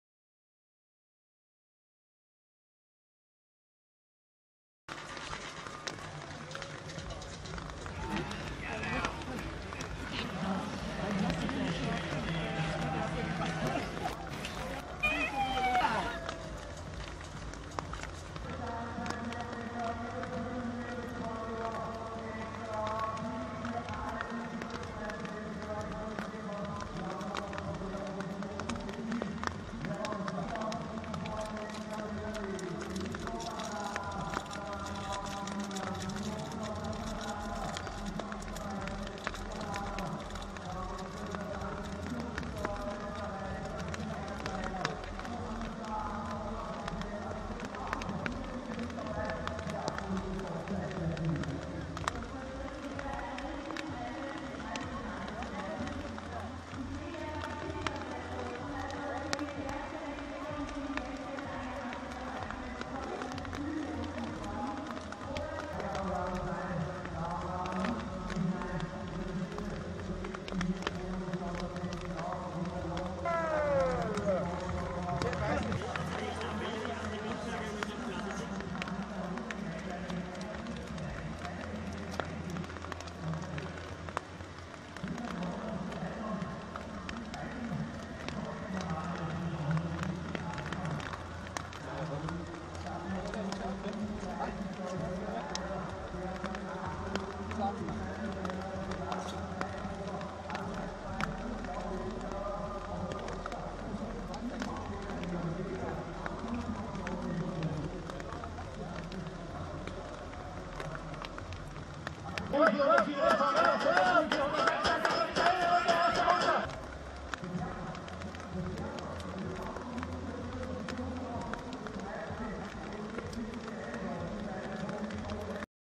Resell climbs to Red Bull 400 metre
.honours on world famous ski jumping hill